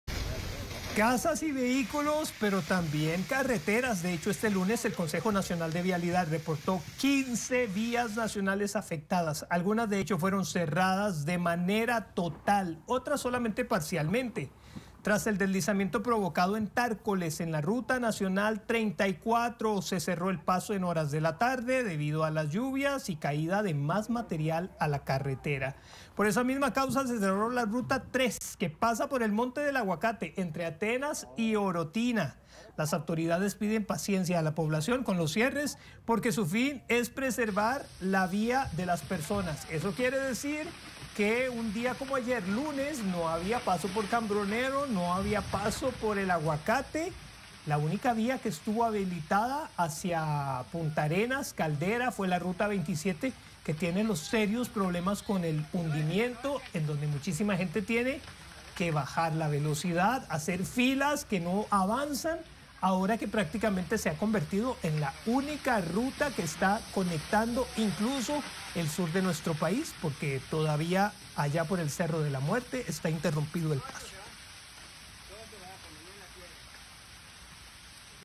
Sector de Tárcoles y Aguacate cerrado
CONAVI reportó 15 vías nacionales afectadas.